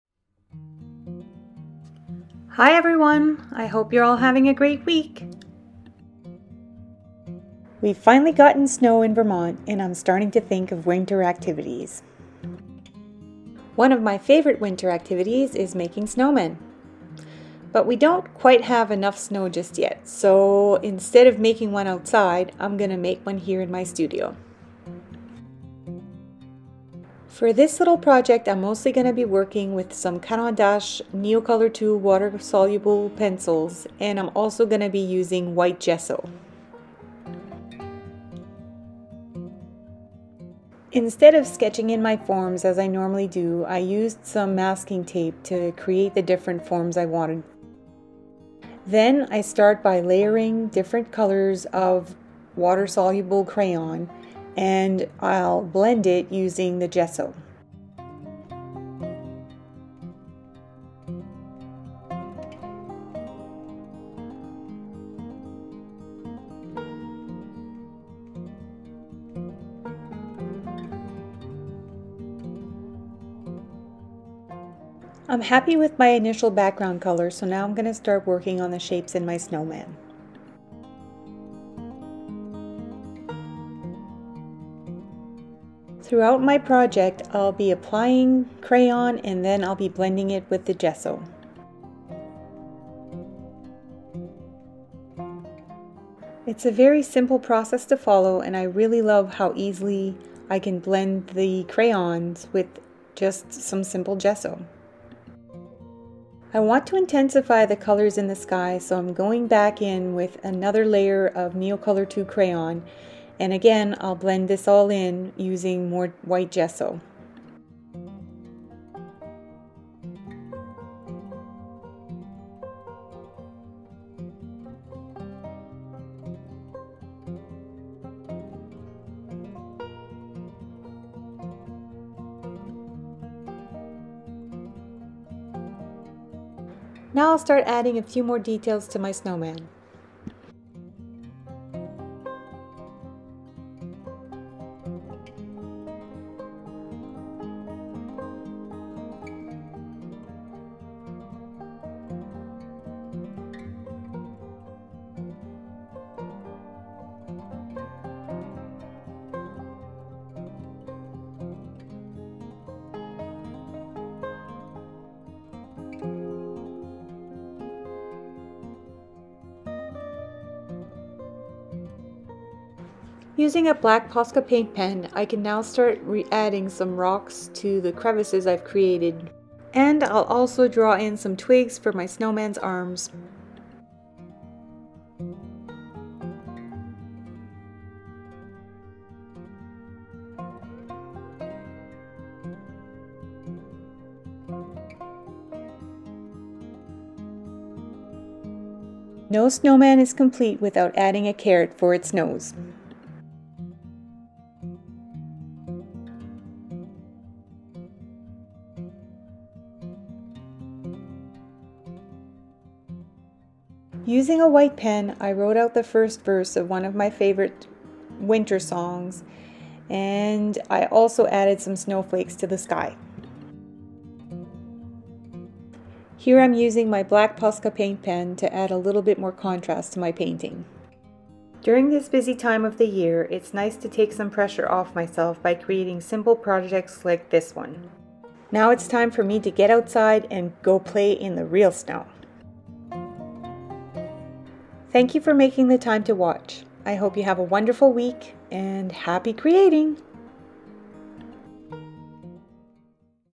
How to Paint a Simple Snowman | Beginner Level Project
This video is a step by step process for painting a fun and whimsical snowman in a winter landscape. I used water soluble crayons, water and gesso to create most of the effects in the painting. It's a fairly simple process to use for beginners.

Here’s a list of the materials I used for this painting:

* watercolor paper
* artist tape or masking tape
* neocolor II water-soluble crayons
* water
* paper towels
* white gesso
* Black Posca paint pen
* White pen
* round and filbert paint brushes of varying sizes

I recorded my video using my iPhone 12 and edited my video using iMovie.

Please don’t hesitate to ask questions or post comments! I'd love to hear from you.

Gabrielle :)